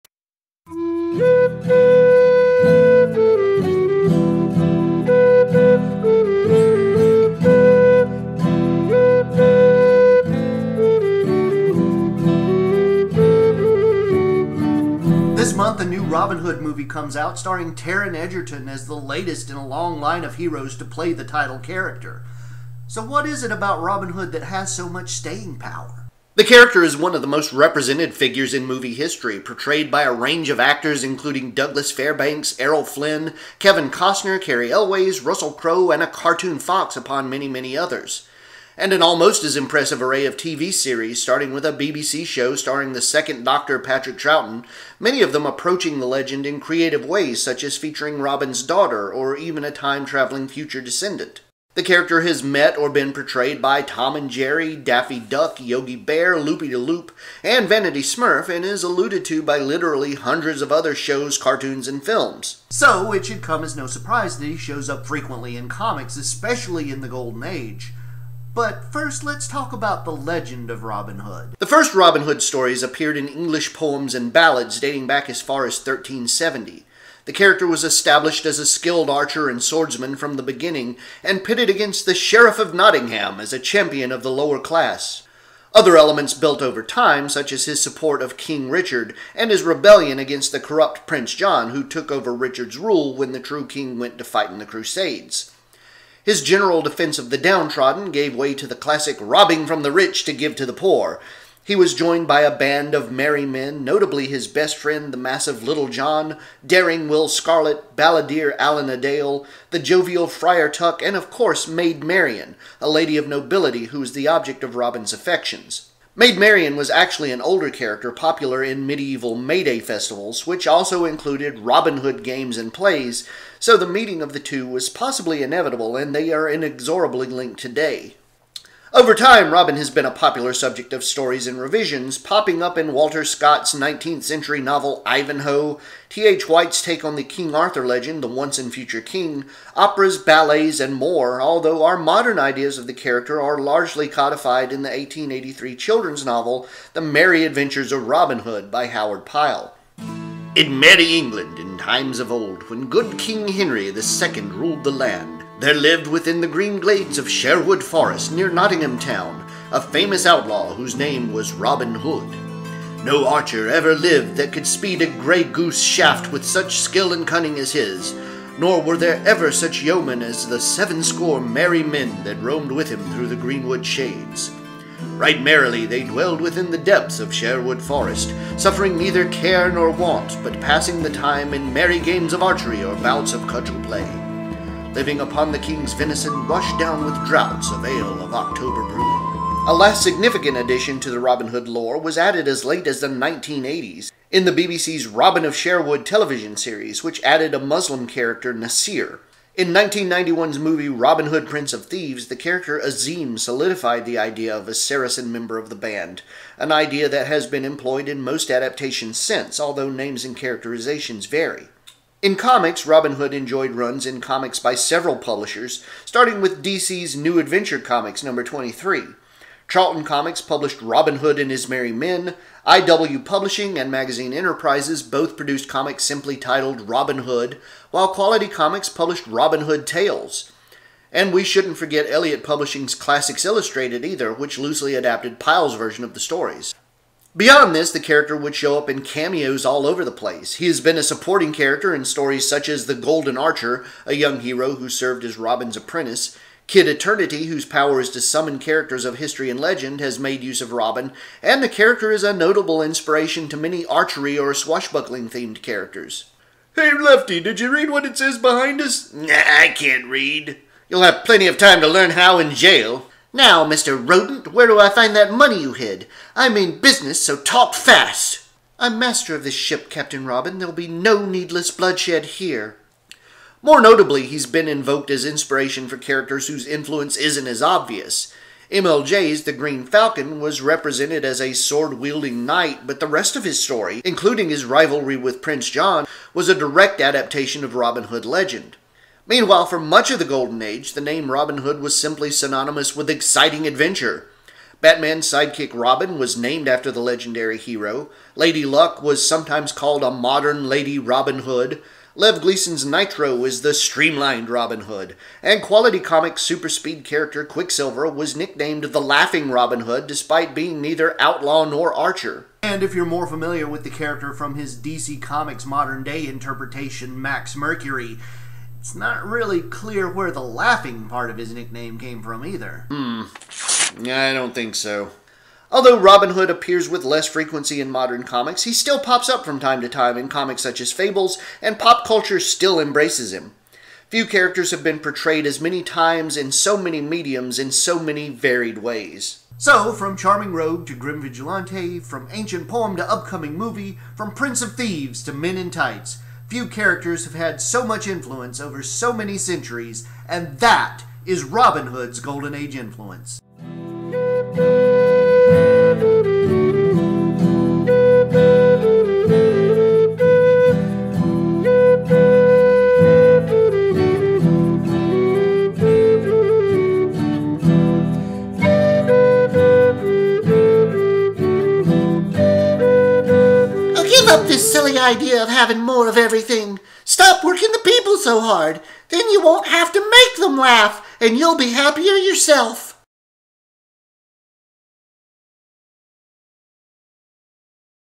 Golden Age Influences: Robin Hood and the Golden Age of Robin Hood
NOTE: I state that Robin of Sherwood (1984) was a BBC show. It was actually produced by ITV. Sorry for any confusion.

Besides being one of the most represented figures in movie history, Robin Hood has also appeared in his fair share of comics. So what is it with 1950s comic creators and their clean-shaven Robin Hoods? It just don't look right.

"Wow" Kid frame by David Cummings